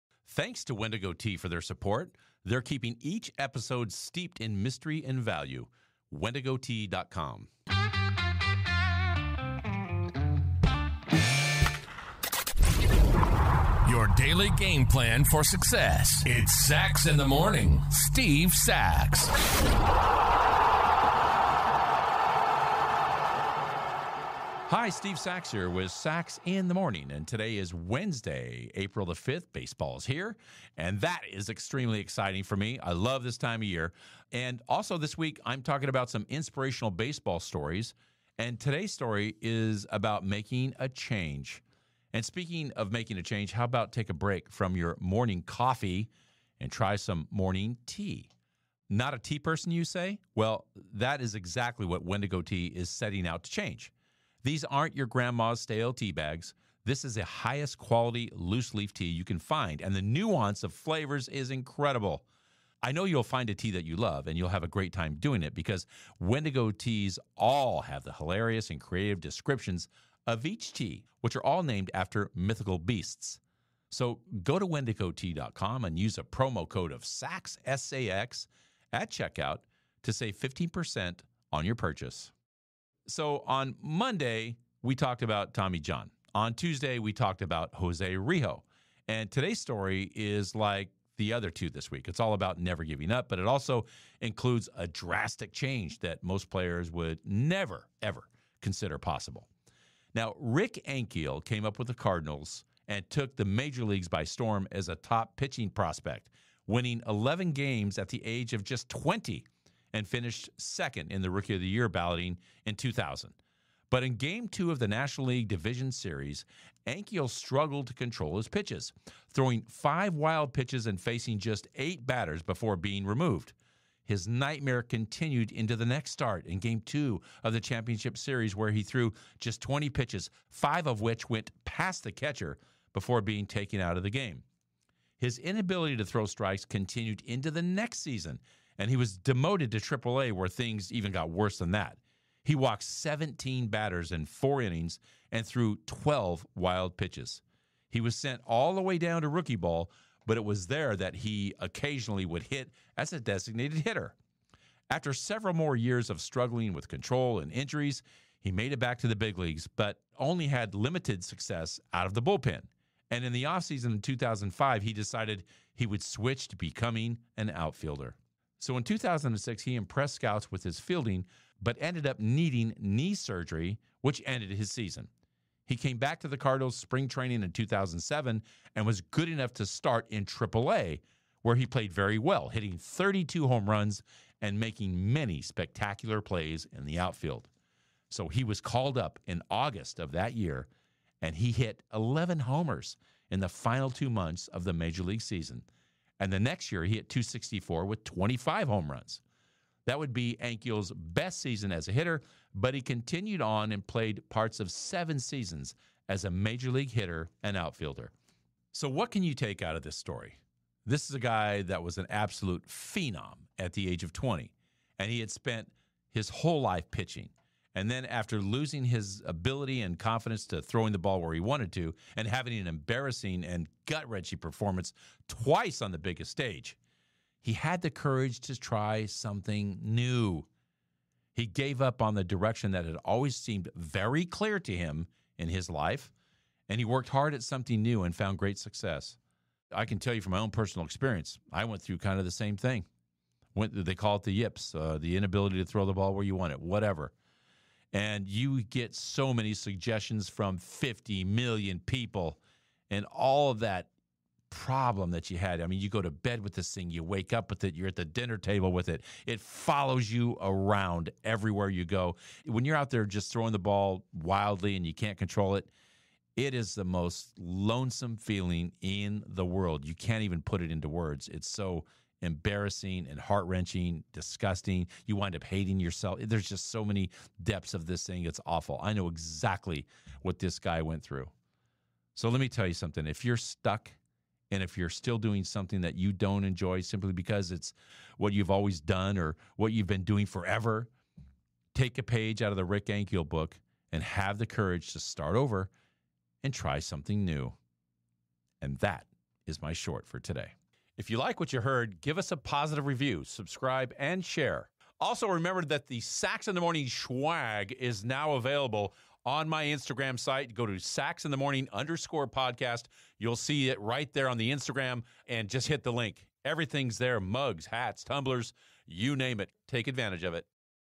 Sax in the Morning: Rick Ankiel had one of the most unusual baseball careers
Today I tell the story of Rick Ankiel who was a pitcher for the St. Louis Cardinals and who rocketed to stardom in 2000. But he fell into baseball ruin nearly as fast as he rose to stardom developing control problems and falling apart in the playoffs of that year.

Ankiel had years of injuries and attempts to solve his control problems before he gave up on his lifelong ambition of being a pitcher and decided to become an outfielder.

In 2007 he torched the minor leagues hitting 32 home runs by August and was called up to the majors where he spent the next seven seasons as an outfielder, having a fine career.

So if you are stuck in a rut or failing over and over at something you know isn’t right for you, have the courage to change your course and start something new.